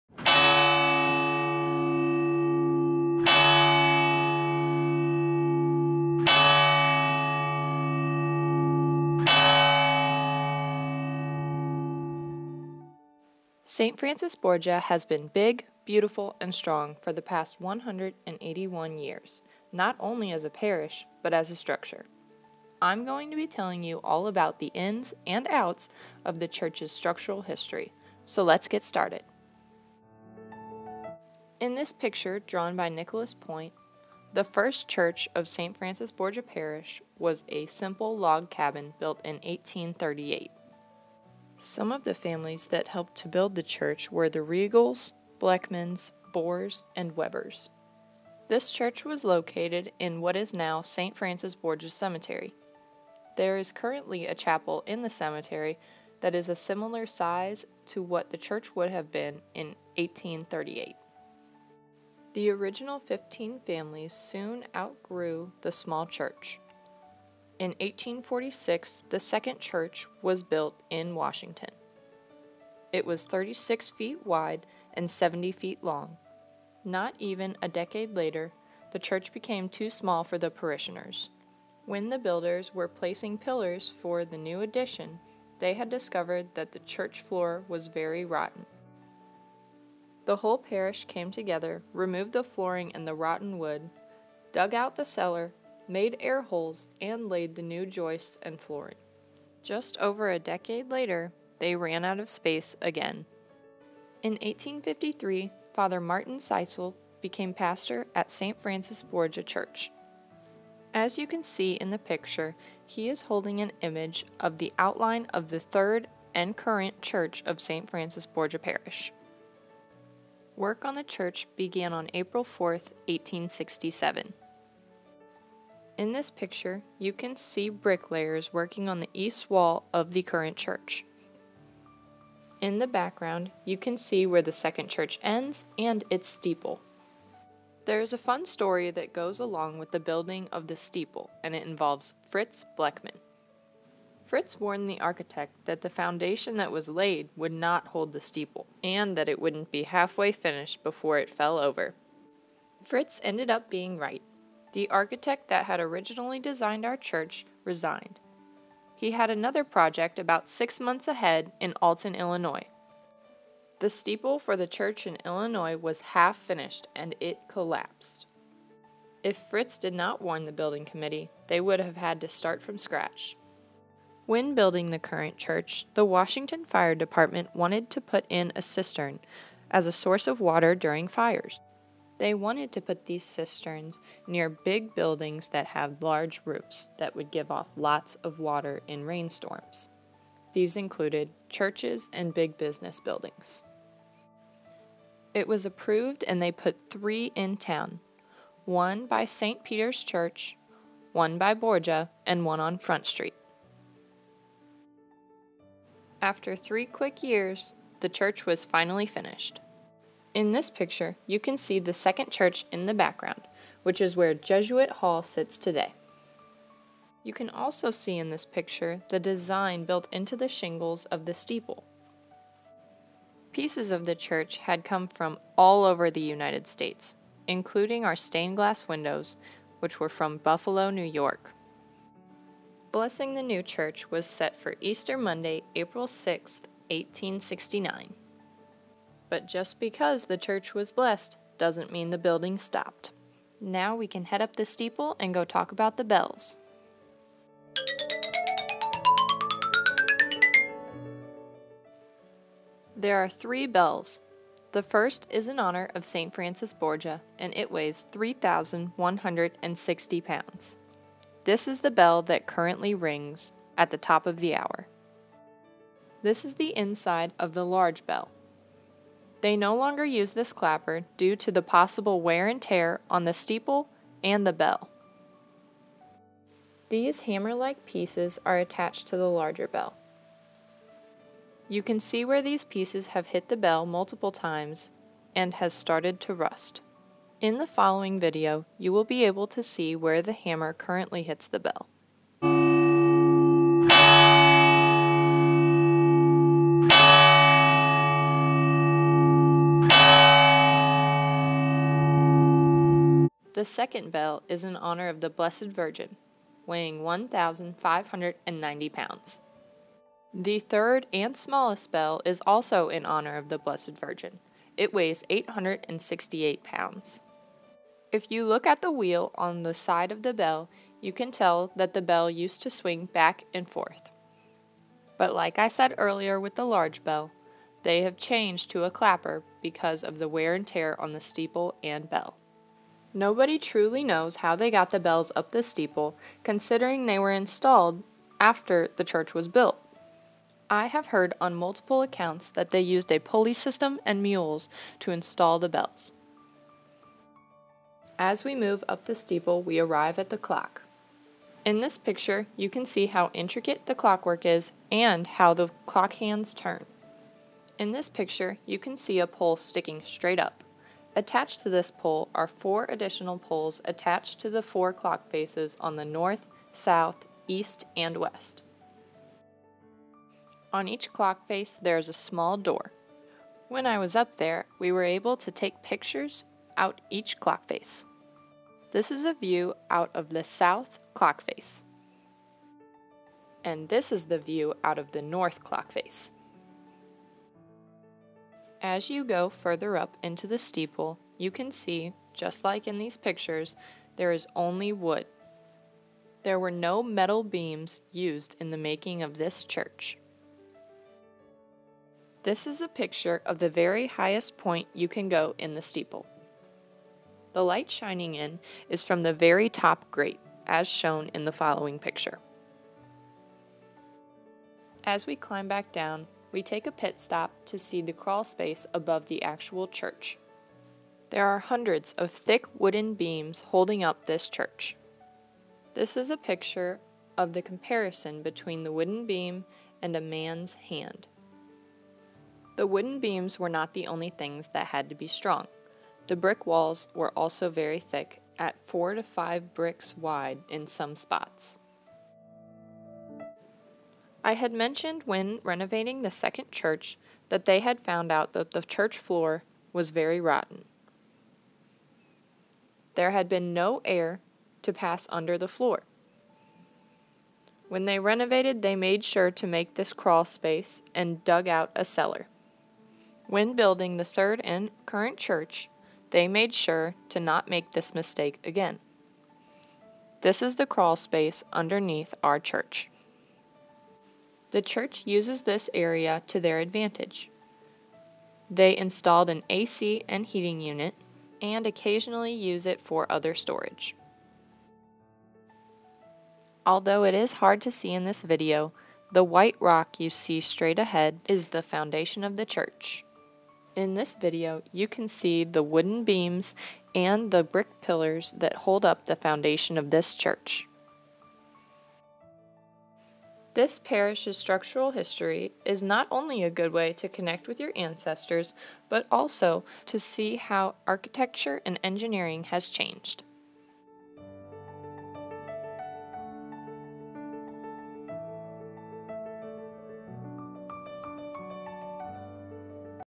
St. Francis Borgia Church by Sophie Straatmann
Sophie was the 2019 St. Francis Borgia Regional High School winner of the Stanley and Marjorie Wilke Memorial Fund Scholarship presented by the Washington Historical Society